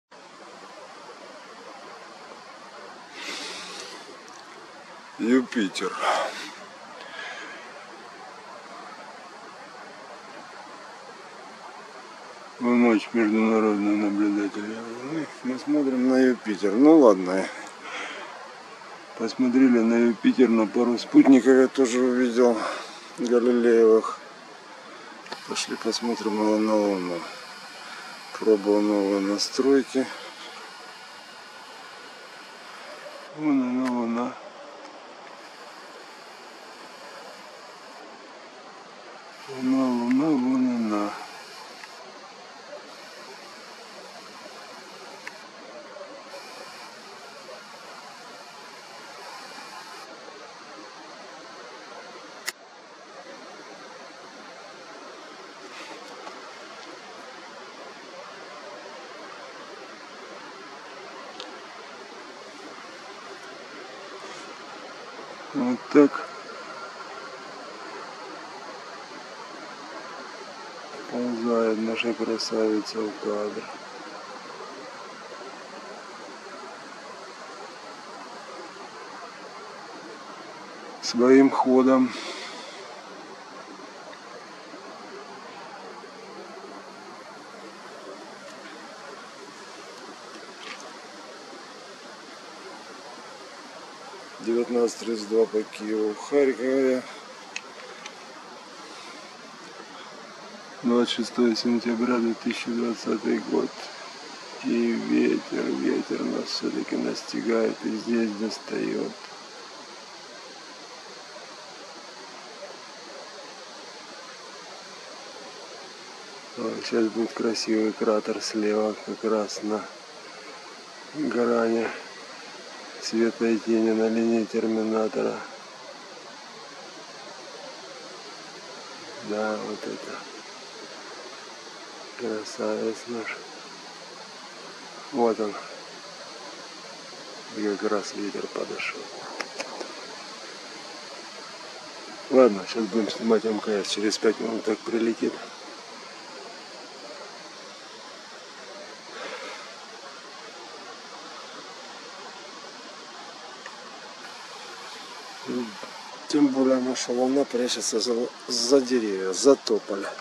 Луна и Юпитер 26 сентября 2020
Луна и Юпитер со спутниками 26 сентября 2020 года над Харьковом.